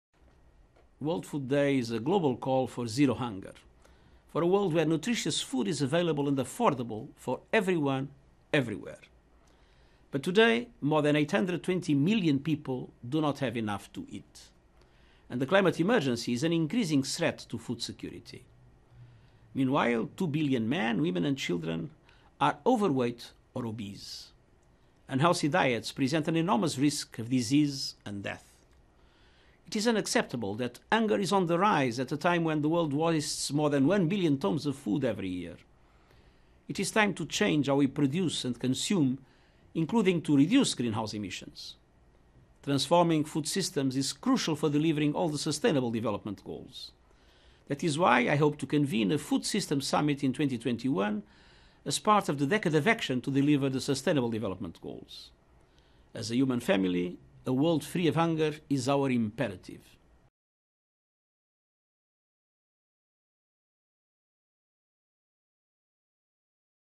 महासचिव एंतोनियो गुटेरेश - विश्व खाद्य दिवस पर (16 अक्तूबर 2019)
विश्व खाद्य दिवस पर संयुक्त राष्ट्र महासचिव एंतोनियो गुटेरेश का वीडियो संदेश.

विश्व खाद्य दिवस भुखमरी मिटाने के लिए वैश्विक पुकार है – एक ऐसी दुनिया बनाने के लिए जहाँ पौष्टिक भोजन हर जगह, सभी को मयस्सर हो.

लेकिन आज, 82 करोड़ से ज़्यादा आबादी के पास पेट भरने के लिए पर्याप्त भोजन उपलब्ध नहीं है.

और जलवायु आपदा खाद्य सुरक्षा के लिए लगातार बढ़ता ख़तरा है.

दूसरी तरफ़, दो अरब पुरुष, महिलाएँ और बच्चे या तो मोटापे के शिकार हैं या उनका वज़न बढ़ा हुआ है.

ख़राब ख़ुराक की वजह से बीमारियों व मौत का भारी ख़तरा होता है.

ये कैसे स्वीकार्य हो सकता है कि भुखमरी का सामना कर रहे लोगों की संख्या लगातार बढ़ रही है, दूसरी तरफ़ हर साल दुनिया भर में एक अरब टन से भी ज़्यादा भोजन बर्बाद कर दिया जाता है.

अब हमें अपनी खाद्य उत्पादन व खान-पान की आदतें बदलनी होंगी, जिनमें ग्रीन हाउस गैसों का उत्सर्जन कम करना भी शामिल हो.

सभी टिकाऊ विकास लक्ष्य हासिल करने के लिए खाद्य व्यवस्था को बदलना बहुत ज़रूरी है.

इसीलिए मैंने 2021 में खाद्य प्रणाली सम्मेलन आयोजित करने की आस लगाई है. ये टिकाऊ विकास लक्ष्य हासिल करने के लिए कार्रवाई दशक का हिस्सा होगा.

मानव समाज का हिस्सा होने के नाते, भुखमरी से मुक्त दुनिया हम सबकी ज़रूरत है.